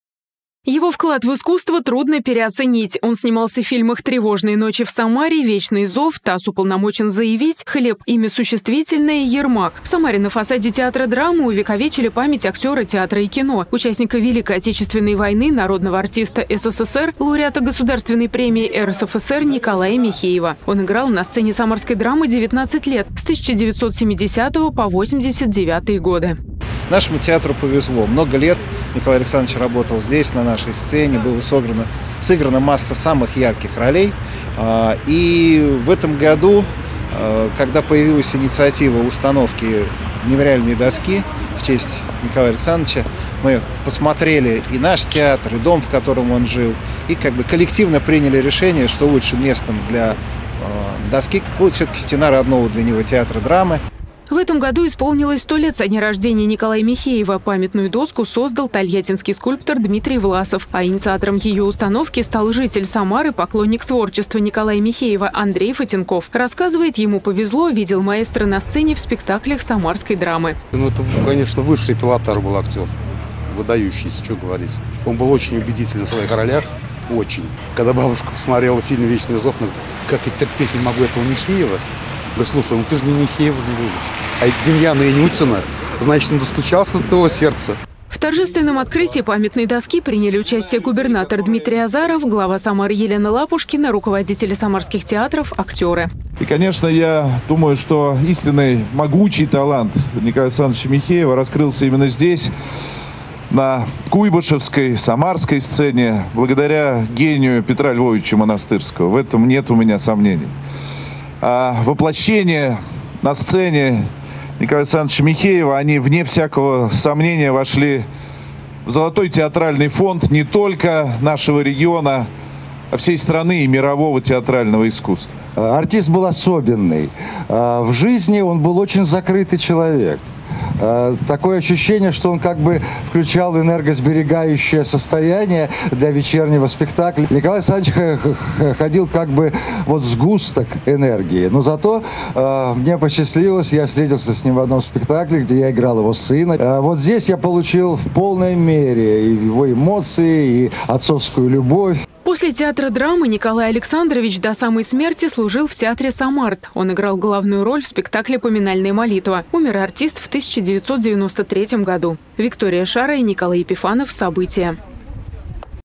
В Самаре открыли мемориальную доску в честь актера Николая Михеева
Гордость всех жителей региона, вклад в наше будущее, защита от исторического беспамятства. В Самаре на фасаде театра драмы появилась памятная доска артисту Николаю Михееву. На торжественном открытии присутствовали губернатор Дмитрий Азаров, глава Самары Елена Лапушкина, руководители самарских театров,  актеры. Николай Михеев снимался в таких культовых фильмах, как "Тревожные ночи в Самаре" и "Вечный зов". И 20 лет играл на сцене самарской драмы. Подробности биографии великого актера театра и кино в нашем сюжете.